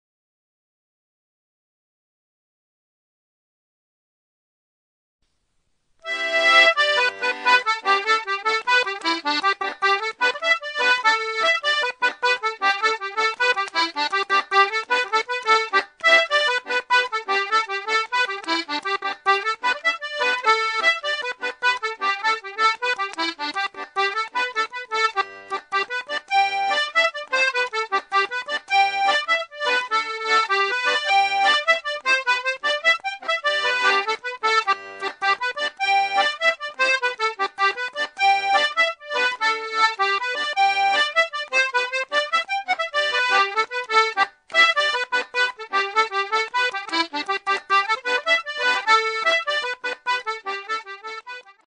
Morris Dancers at the Portman Arms
Dr. Turberville's Morris at the Portman Arms, East Chinnock - 2011